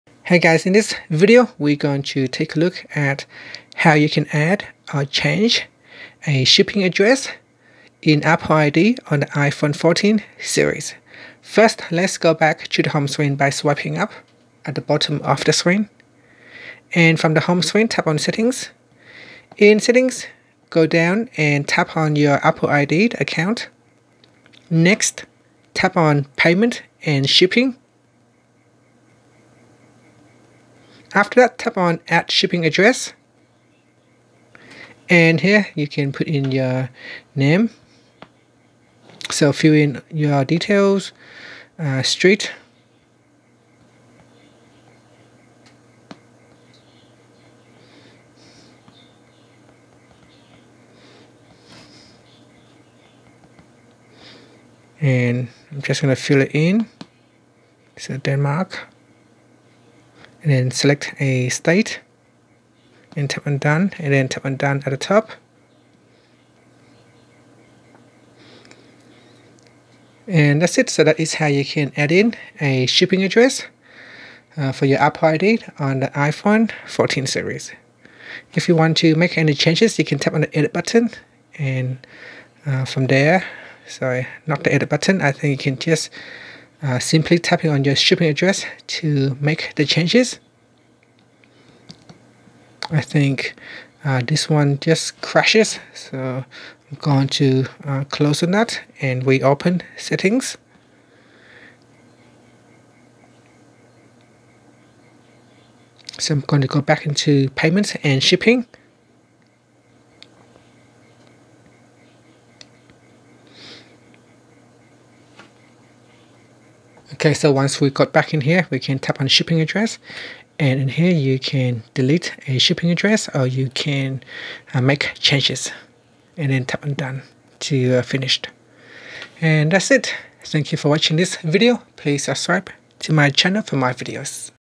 iPhone 14/14 Pro Max: How to Add/Change A Shipping Address In Apple ID
Learn how you can add or change a shipping address in Apple ID on the iPhone 14/14 Pro/14 Pro Max/Plus.